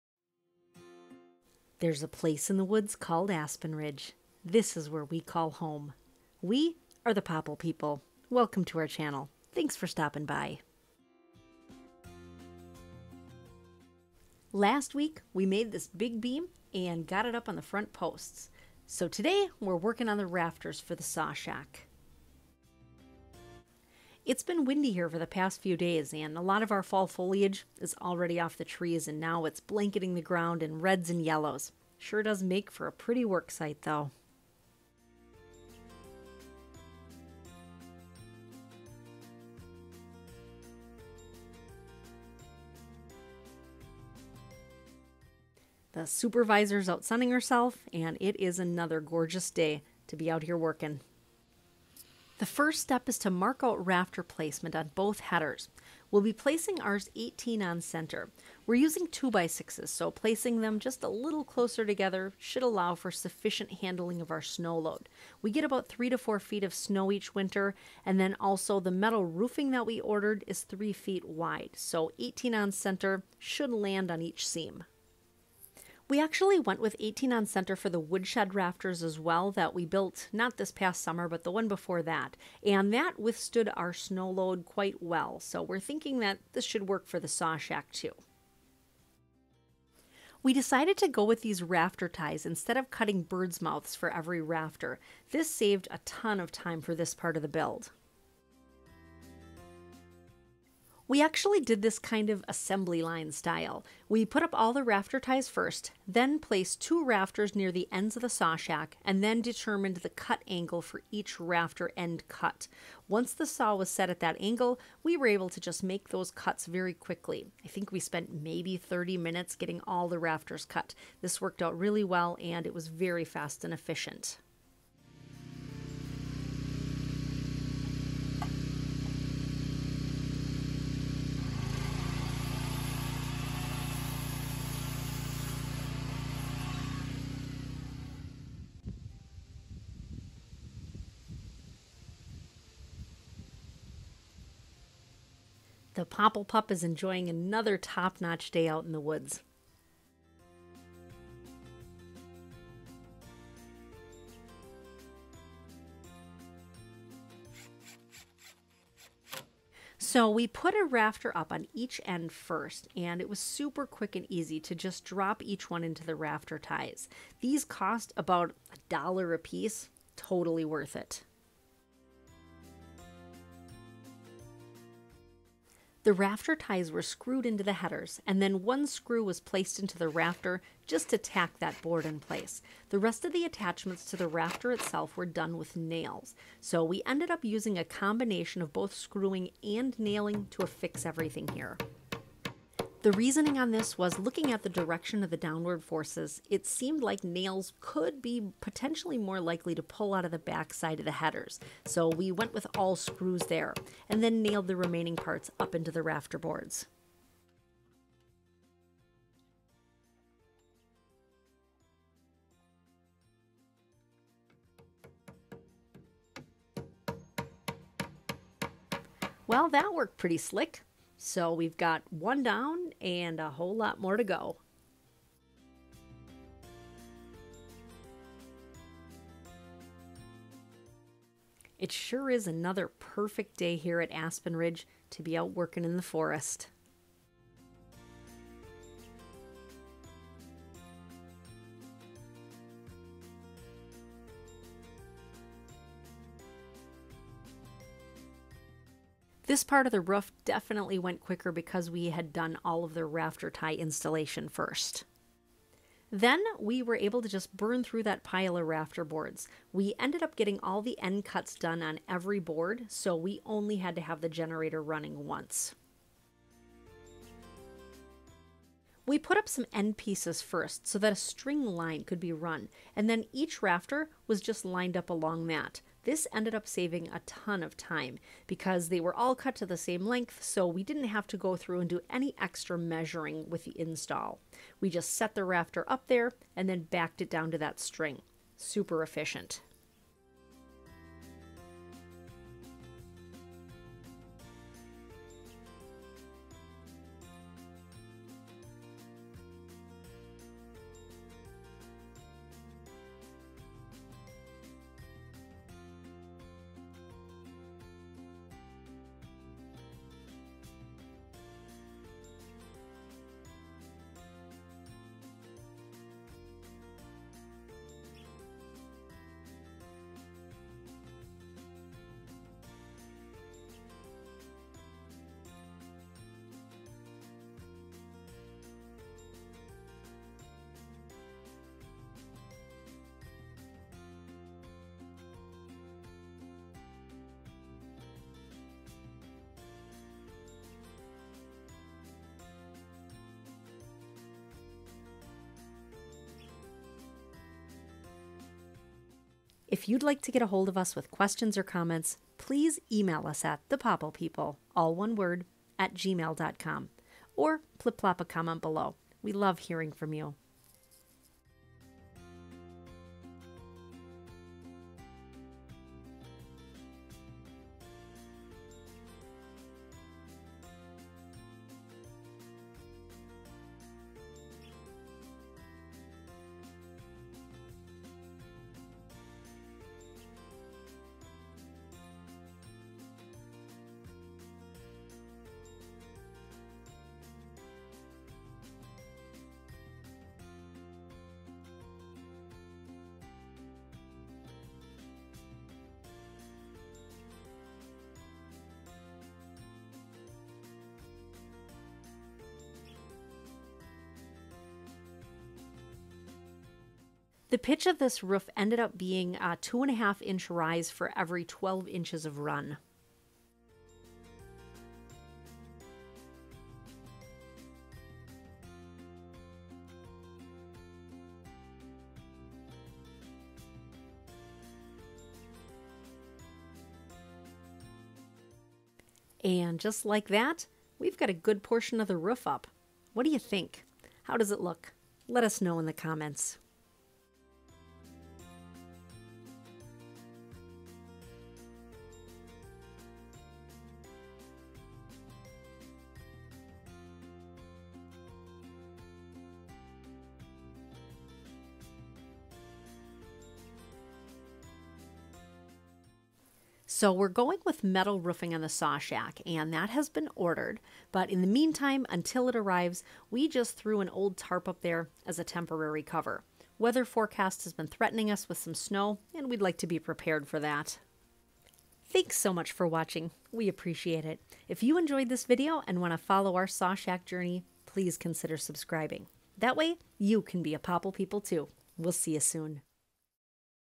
OFF GRID Saw Shack RAFTERS (EASY!)-Popple People-Episode 61
In this episode, we get the rafter boards up…so we *almost* have a ROOF! (If only that metal roofing would arrive….🤔)

This OFF GRID SAW SHACK is all coming together! It’s really starting to look like a building now!

Tune in to see the quick & easy way we installed these rafters!